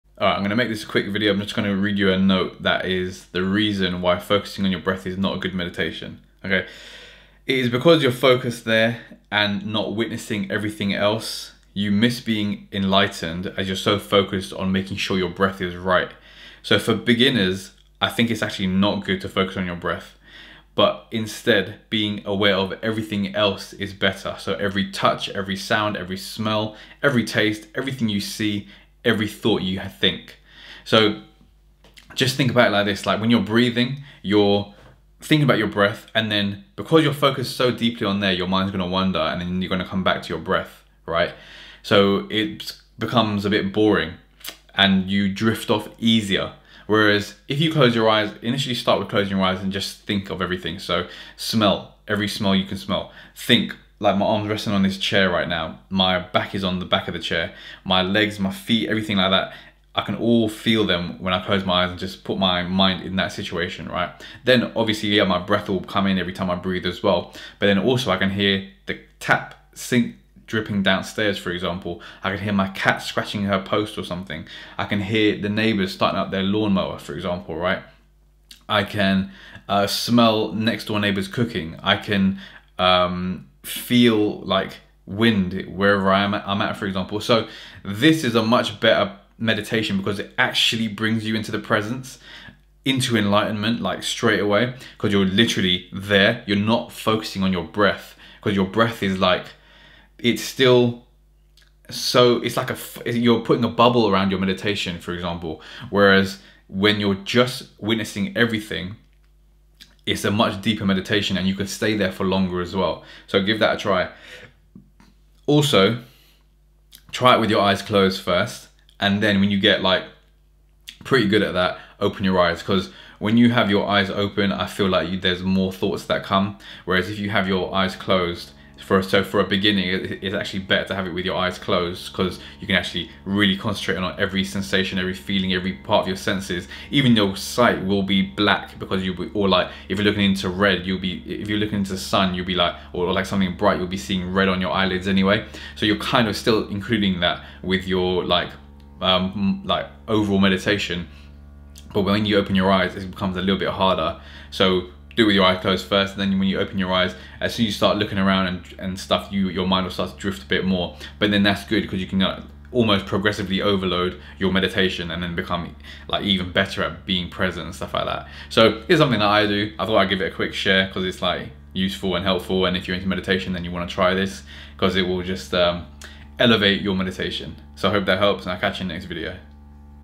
How To Meditate (For COMPLETE Beginners)
A complete beginners guide on how to actually meditate.

Focusing on the breath for beginners takes them away from the actual benefits of meditation as their mind can't relax fully and embrace everything else.

So this method will help to better allow you to rest and relax into your meditation.